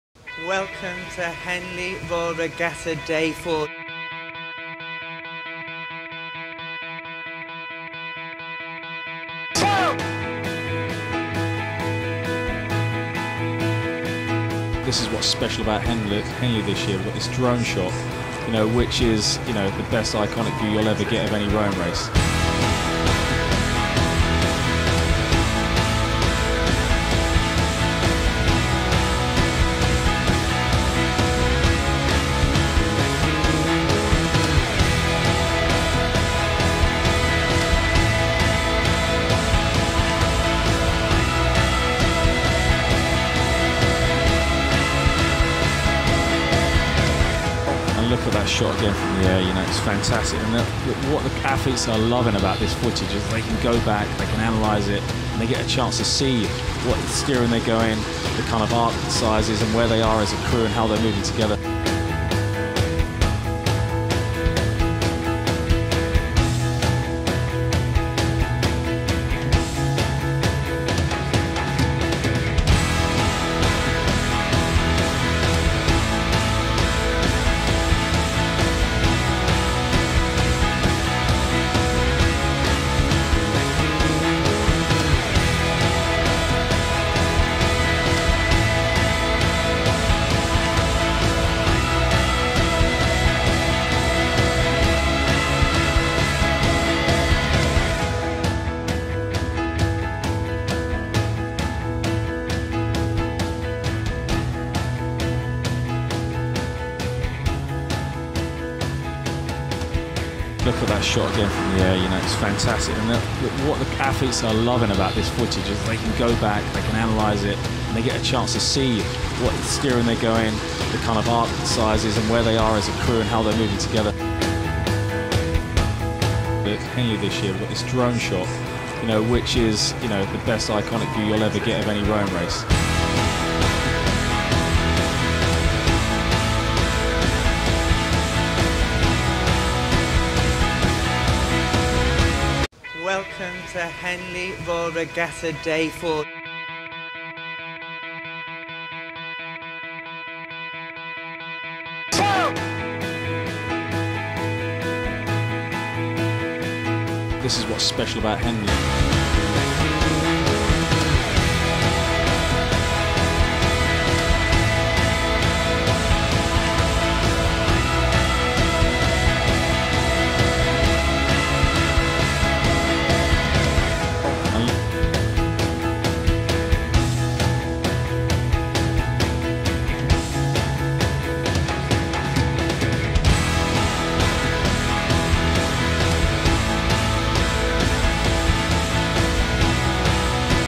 Best of Drone Camera Semi Finals Day | Henley 2015
See rowing from a whole new perspective with this incredible drone camera footage from Semi-Finals day at Henley 2015. Henley Royal Regatta is the .

Relive some of the best moments from the Henley Royal Regatta 2015.

One of the biggest highlights from Day 3 at Henley 2015 was the introduction of the Drone cam to our live coverage, providing incredible camera angles to give .

St. Paul's School v Radley College in the Princess Elizabeth Challenge Cup. As Live Race Replay from Semi-Finals Day at Henley 2015, Saturday 4th July.